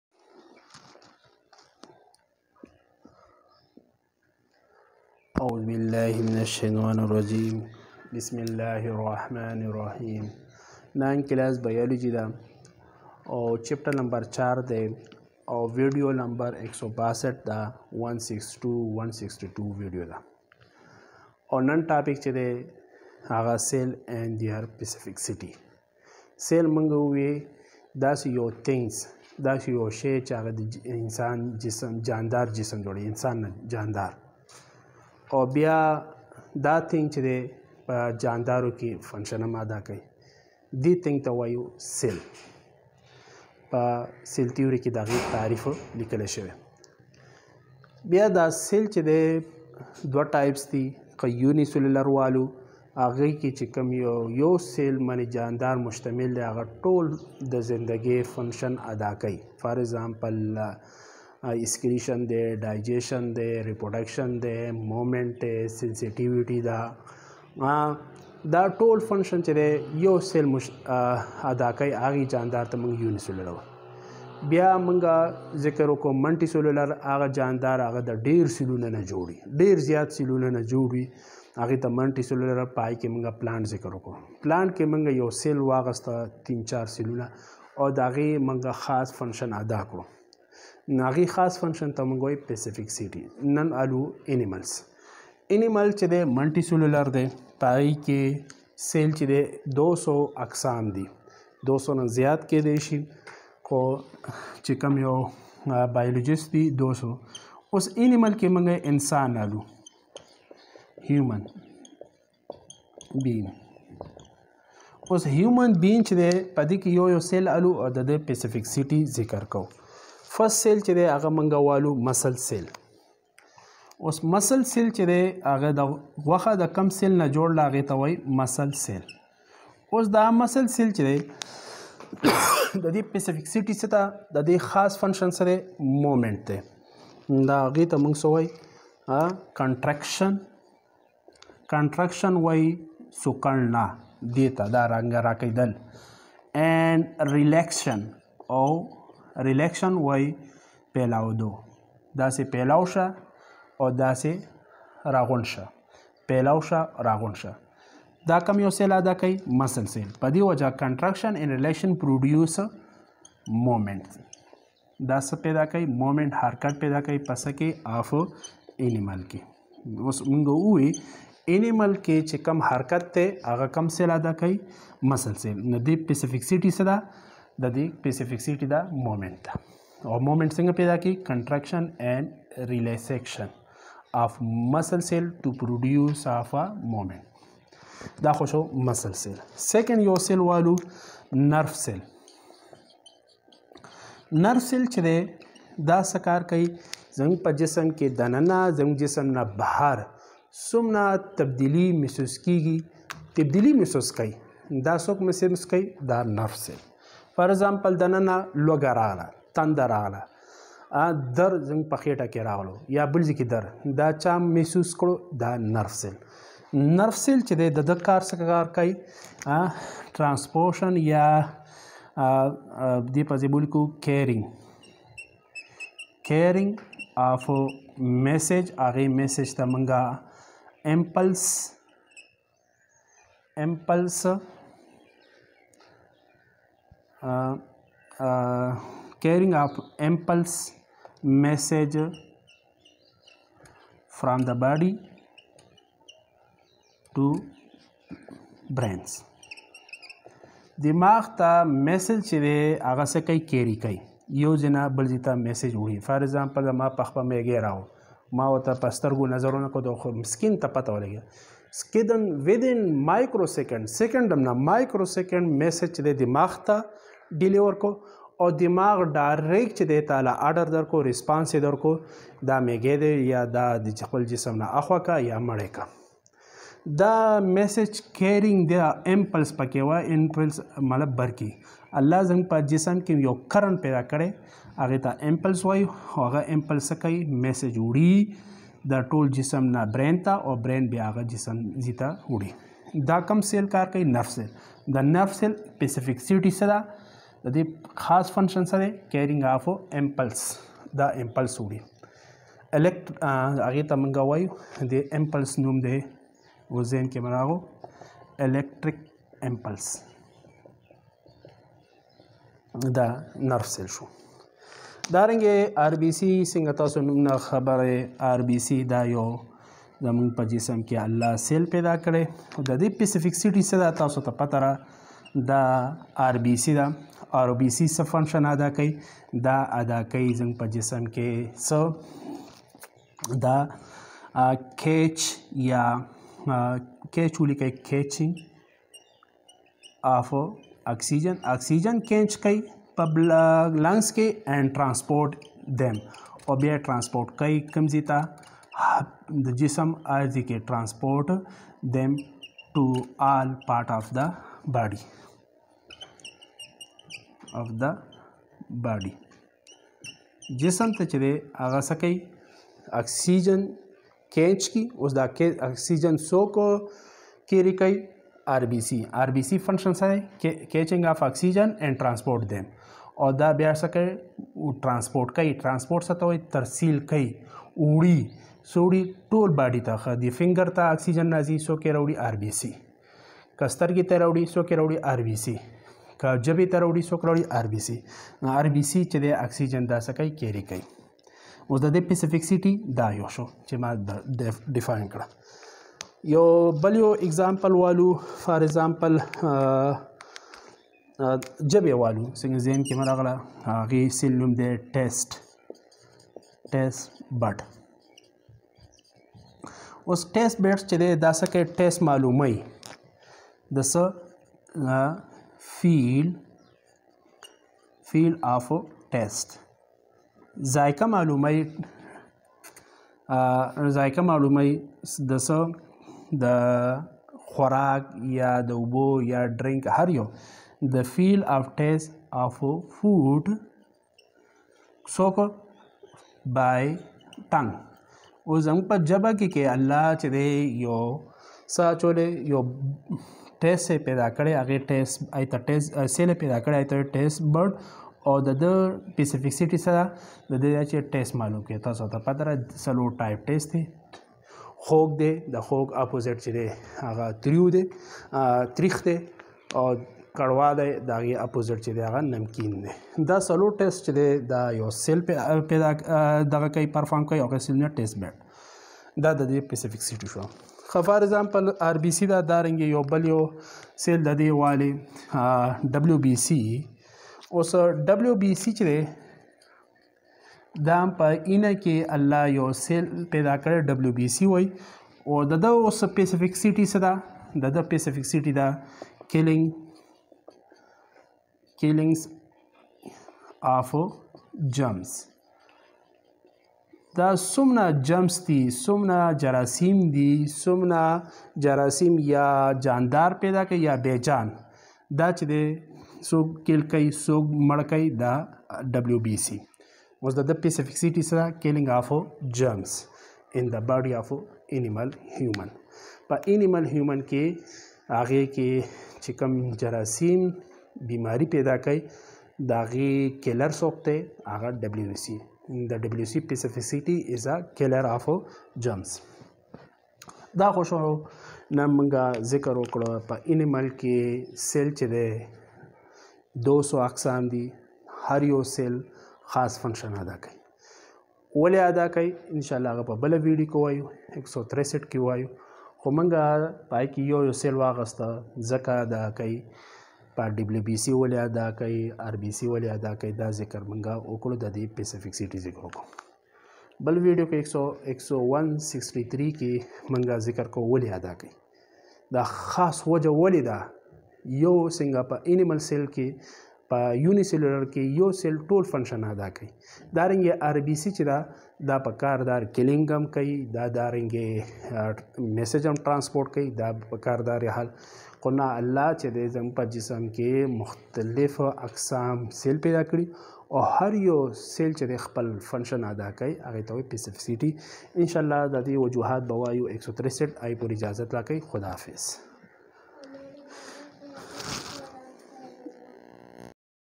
Cell and their specificity | Part 03 | 9th class biology | in Pashto
Cell and their specificity
Every cell perform their specialized function is due to size, shape and presence of organelles. The size are include small, large, long, bulky, volume, area and tiny. the shape may be geometrical that include regular and irregular shapes. the amount of organelles increase in the cell which perform their own specific function.